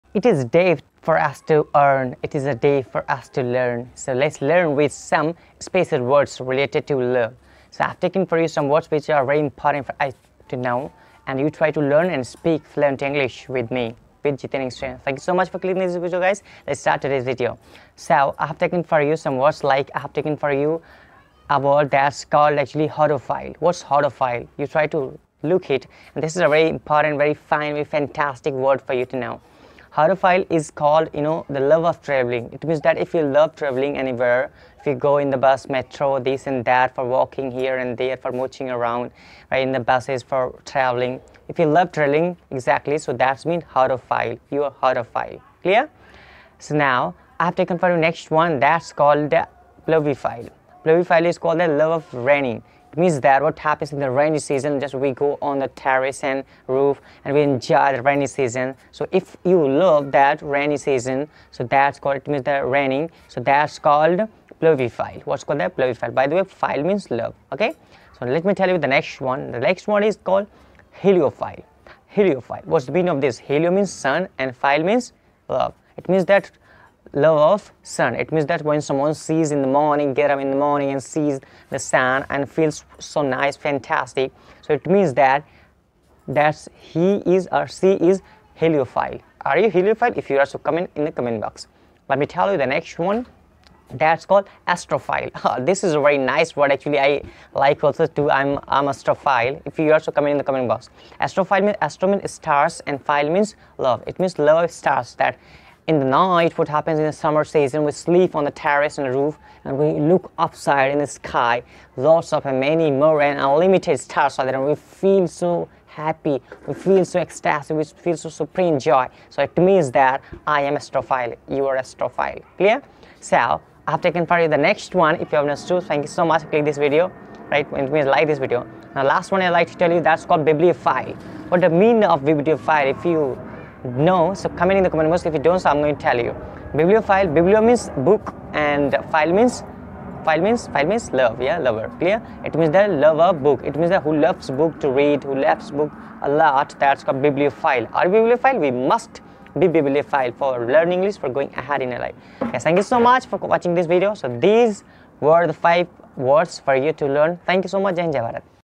phile words | root word phile | daily used english words| english words meaning | Jitendra English
in this video We will learn root words,
phile related words, words framing
1. hodophile
2. pluviphile
3. heliophile
4.bibliophile
etc.
phile words,phile,phile word, learn English

✍️English_speaking
✍️public speaking